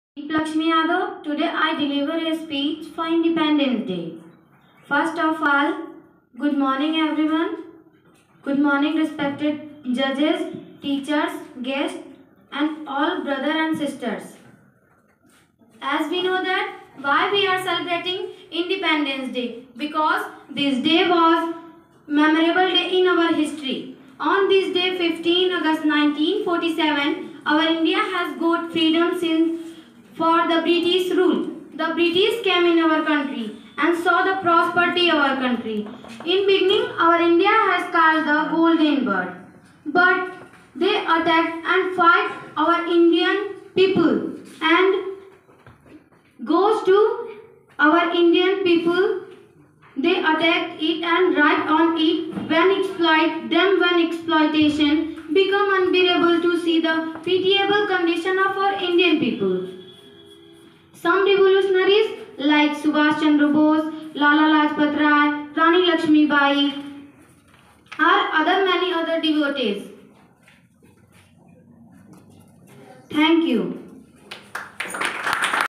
Diplakshmi Speeched now
Today's performance of Speech in English For One time scholarship Program 2022